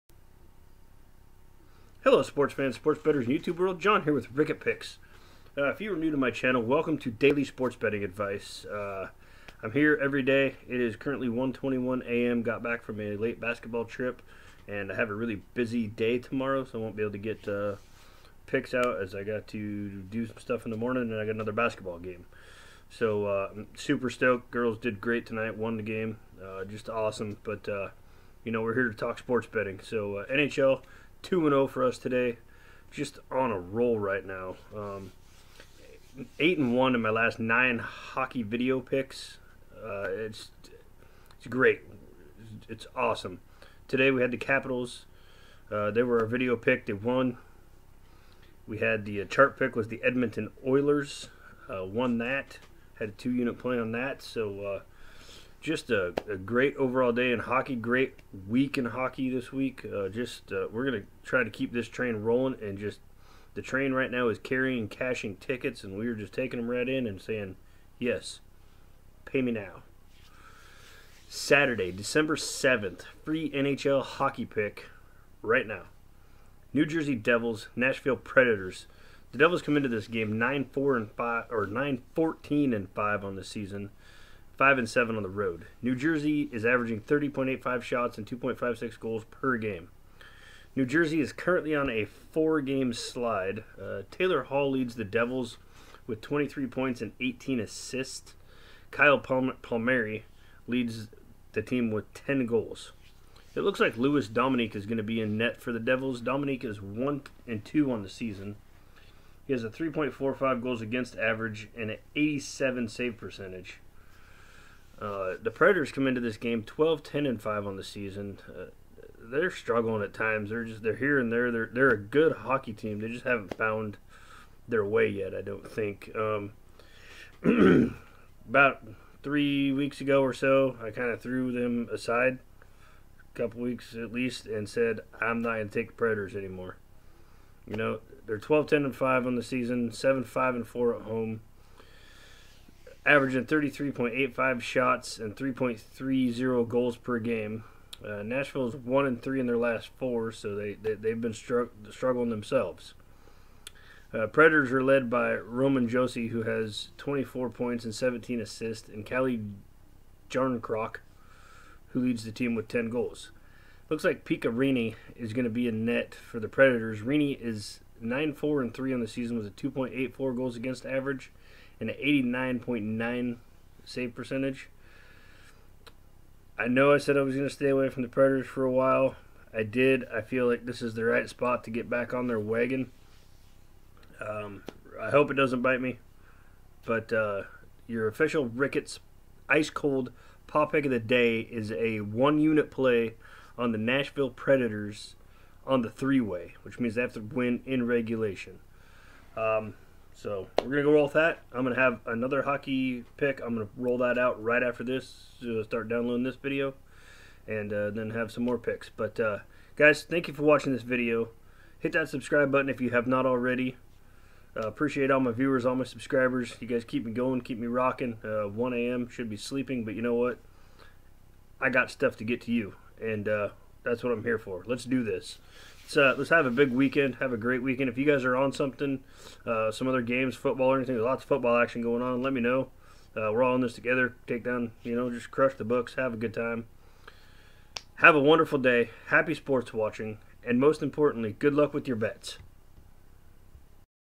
New Jersey Devils @ Nashville Predators, NHL Free Pick, 12-7-19
Daily sports betting advice. NHL Hockey free Picks. 94-62 record. New Jersey Devils @ Nashville Predators. Lets crush the books.